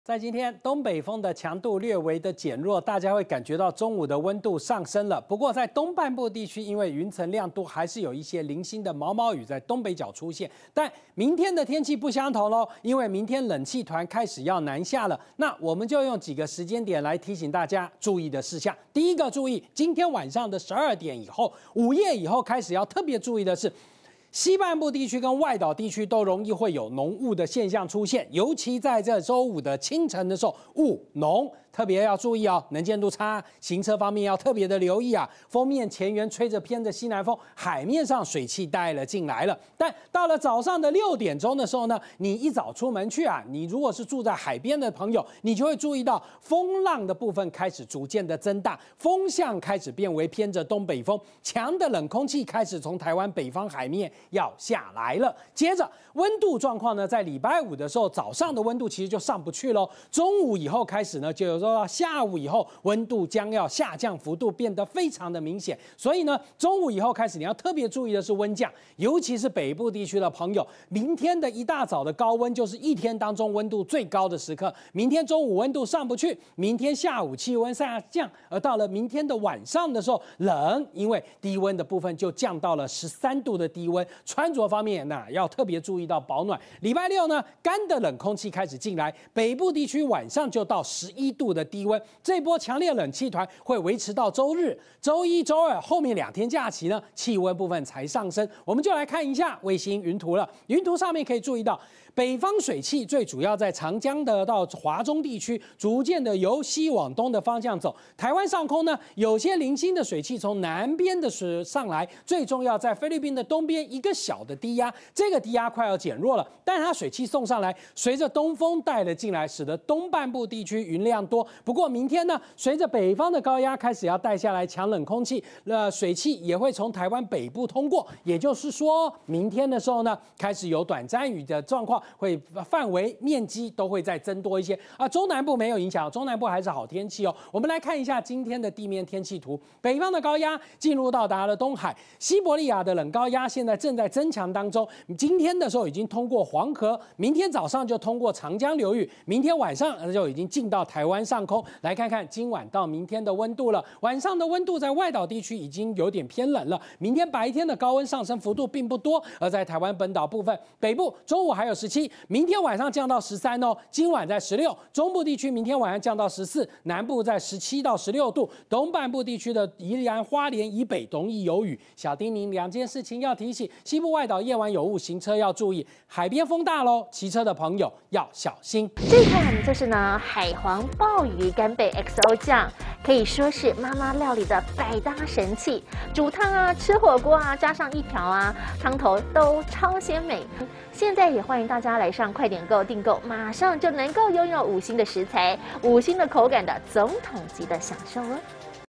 【戴立綱氣象】下週一白天冷空氣減 西部清晨有霧｜強烈冷氣團明天南下 週休低溫11度 2023023
00:00 西部外島夜晚霧 能見度差行車小心
01:02 強烈冷氣團明天南下 週休低溫11度
01:34 下週一白天冷空氣減  西部清晨有霧
01:42 週二天氣晴早晚涼 高山旅遊夜晚冷

推薦影片
劉寶傑直言"郭台銘沒步了沒人了"! 吳子嘉爆內幕"等一個辦法"變"被一個人出賣"?!